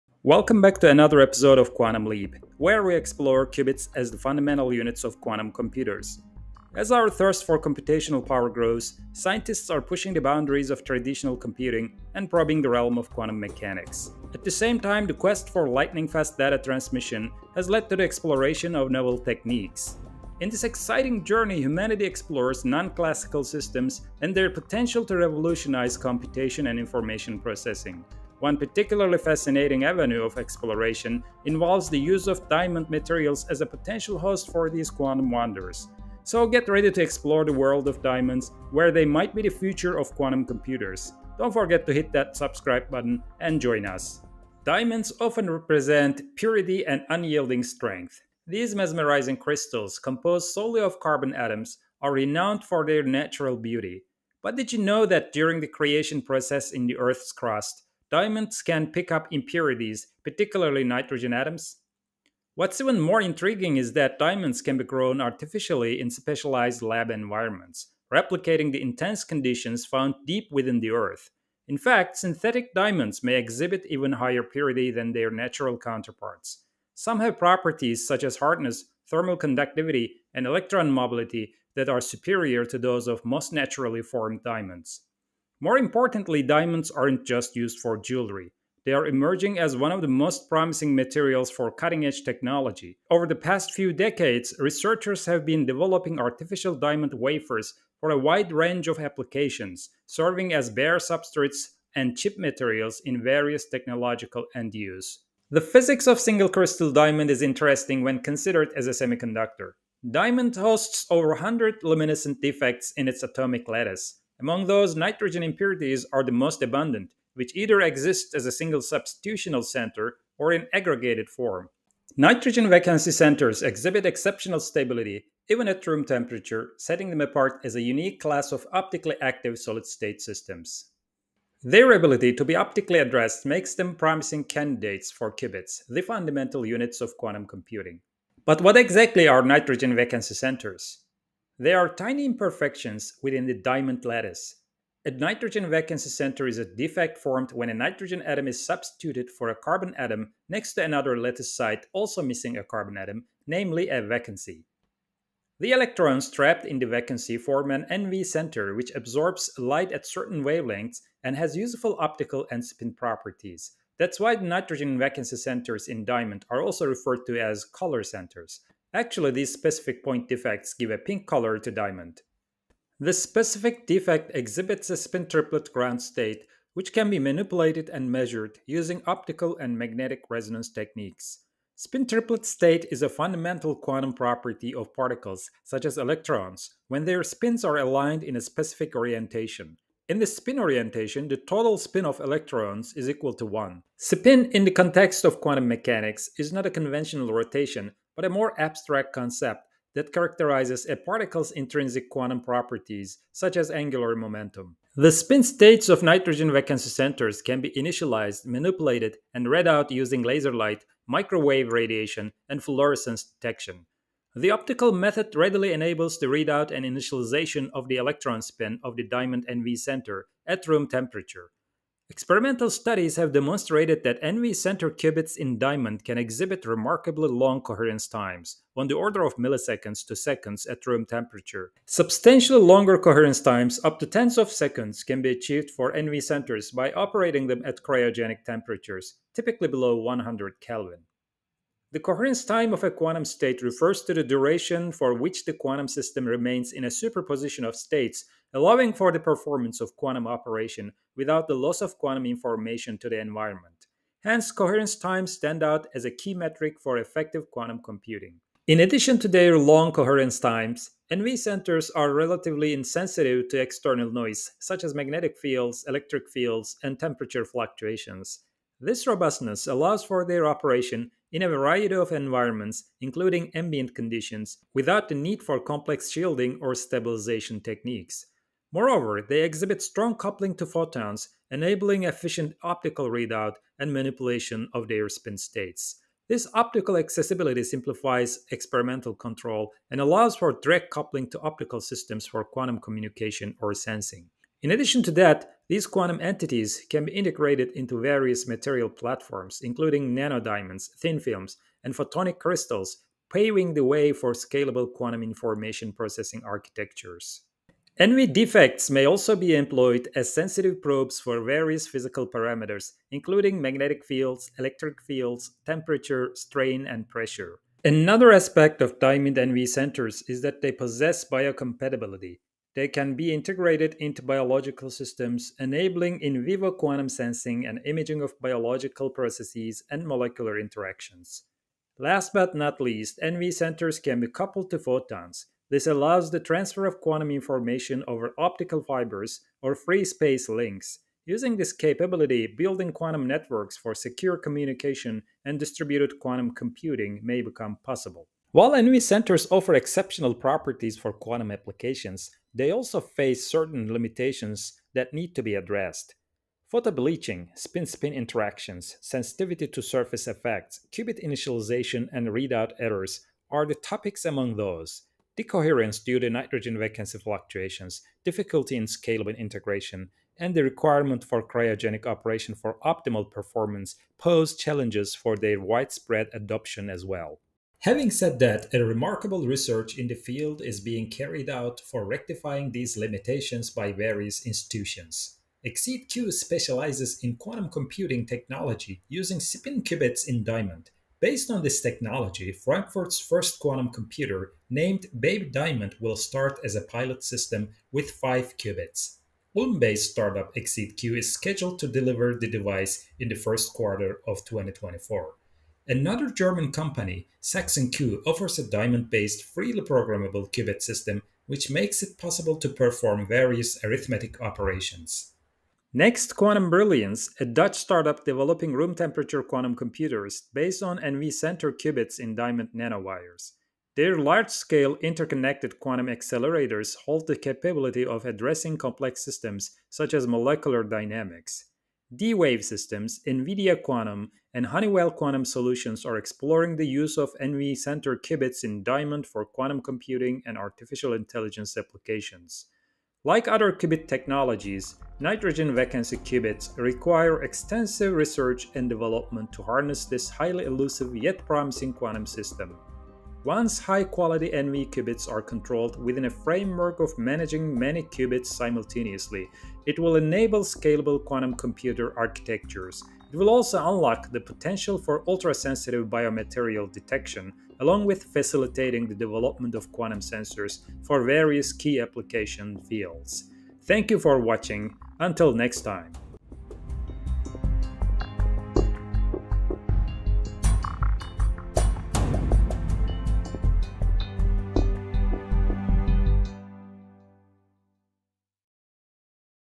Diamond as a Quantum Comupter Building Block
As our thirst for computational power grows, scientists are pushing the boundaries of traditional computing and probing the realm of quantum mechanics. At the same time, the quest for lightning-fast data transmission has led to the exploration of novel techniques.

In this exciting journey, humanity explores non-classical systems and their potential to revolutionize computation and information processing. Researchers are developing theories and experimenting with quantum mechanical systems. One particularly fascinating avenue of exploration involves the use of diamond materials as a potential host for these quantum wonders.

So, get ready to explore the world of diamonds, where they might be the future of quantum computers!